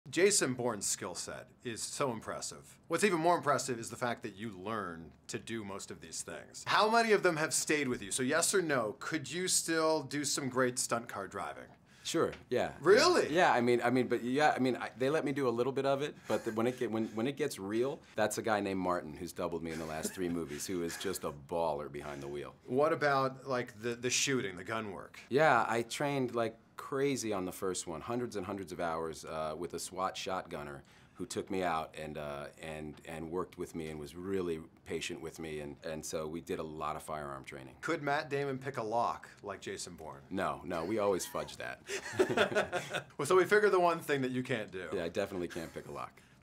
Can Matt Damon ACTUALLY Do Jason Bourne Things In Real Life?
Matt Damon talks to Chris Van Vliet about the skills he learned to play Jason Bourne and whether or not he can actually do any of those things in real life.